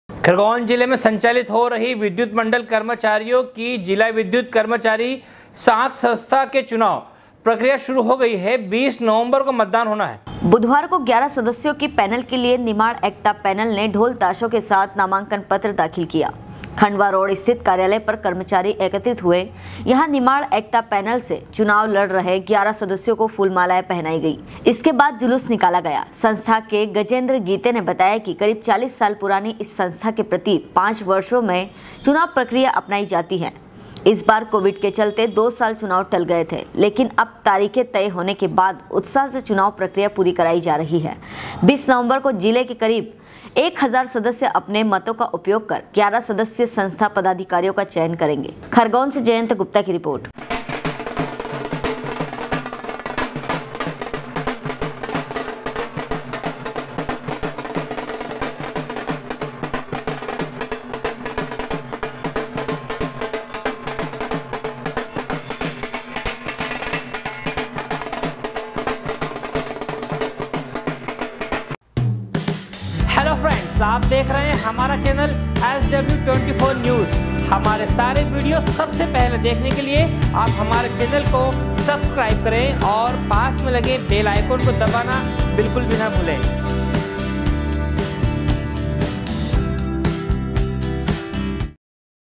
खरगोन जिले में संचालित हो रही विद्युत मंडल कर्मचारियों की जिला विद्युत कर्मचारी साख संस्था के चुनाव
खरगोन जिले में संचालित हो रही विद्युत मंडल कर्मचारियों की जिला विद्युत कर्मचारी साख संस्था के चुनाव प्रक्रिया शुरू हो गई है। 20 नवम्बर को मतदान होना है।
खरगोन से जयन्त गुप्ता की रिपोर्ट।